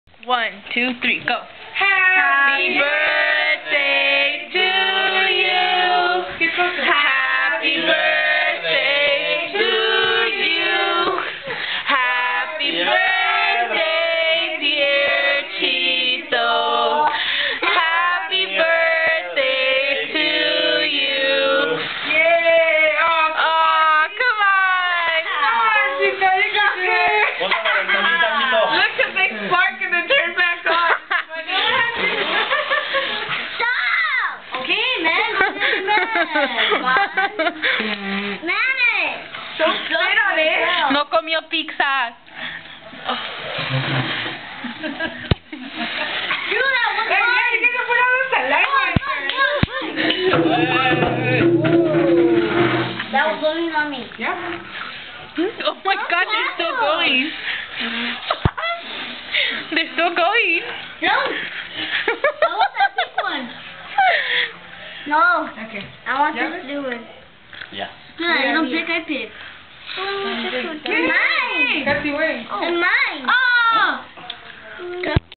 Nat's Birthday again
we couldnt blow the candles from the cake they keeped on coming back lol